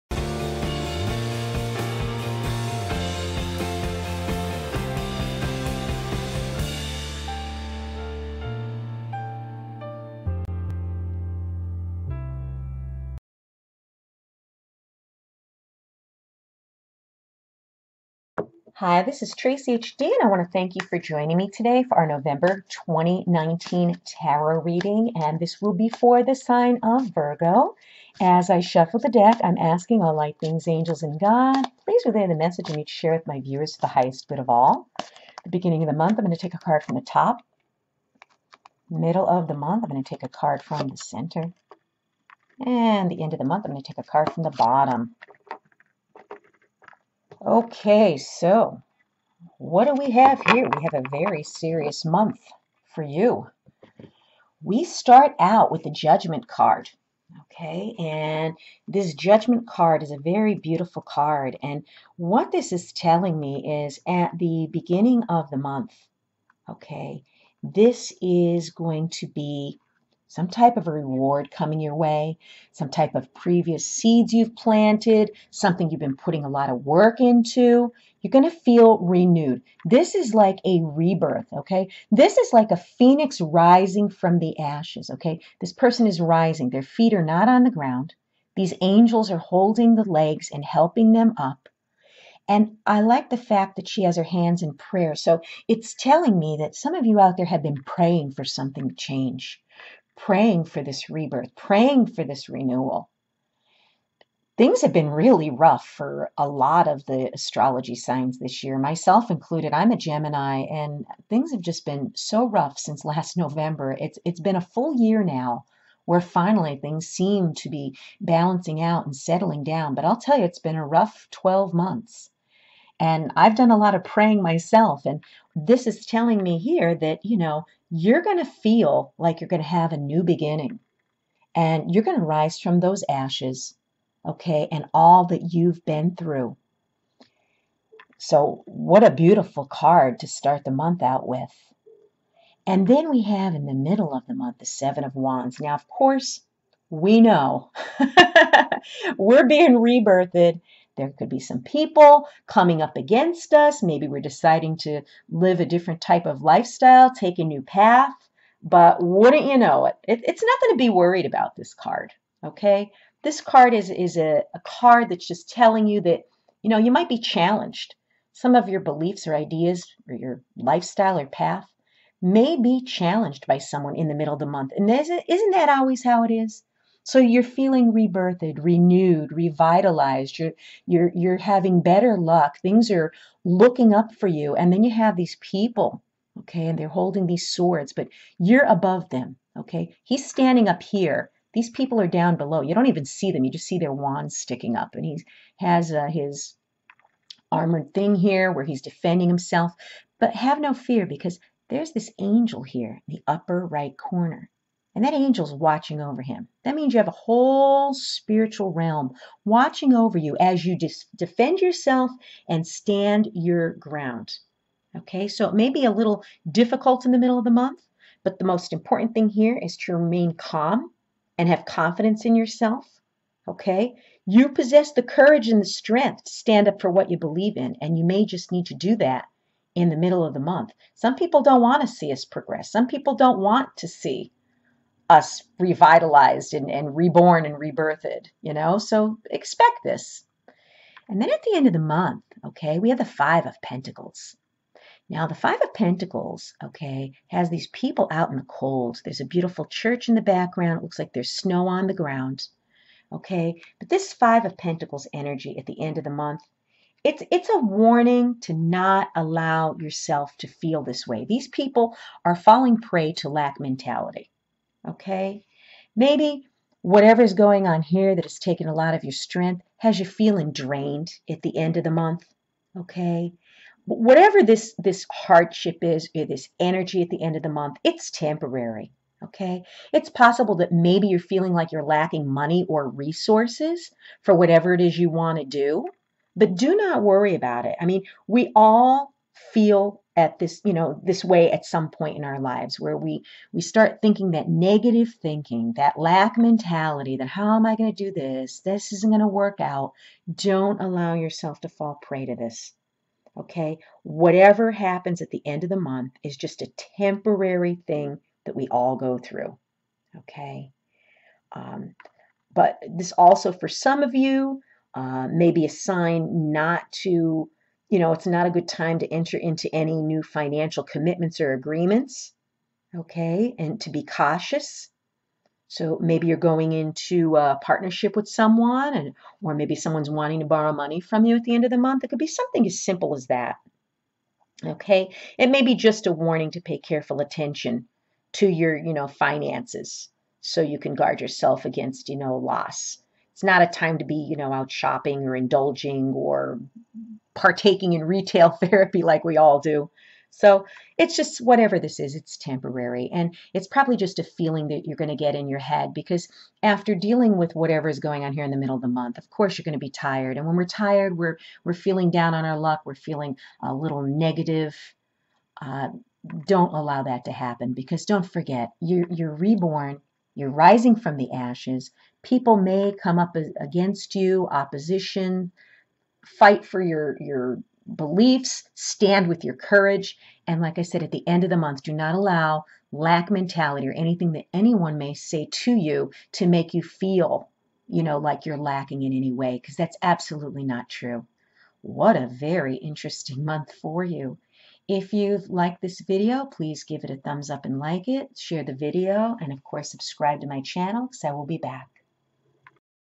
VIRGO Nov 2019 Tarot A PHOENIX RISING FROM THE ASHES
Traceyhd gives the Nov 2019 monthly intuitive tarot astrology love horoscope for VIRGO. She is using the Influence Of The Angels Tarot by Jody Boginski Barbessi & Karen Bogonski. What should we expect? Watch & find out.